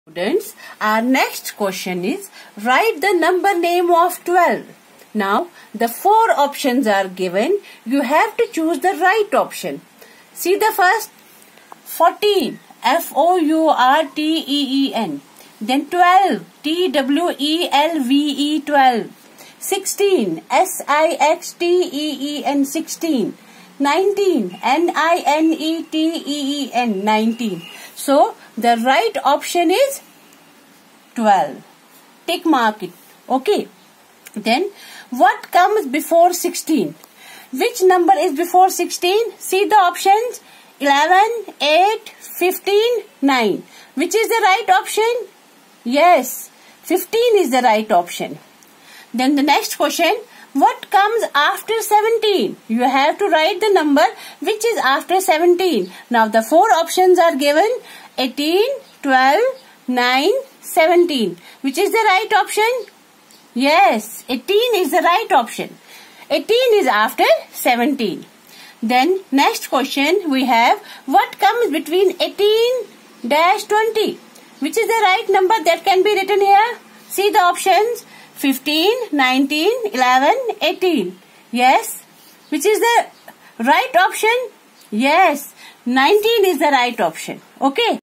Class 1st (Maths) : Revision - Part 1 Continued...
In this video also we will learn number names, what comes before-after, what comes between.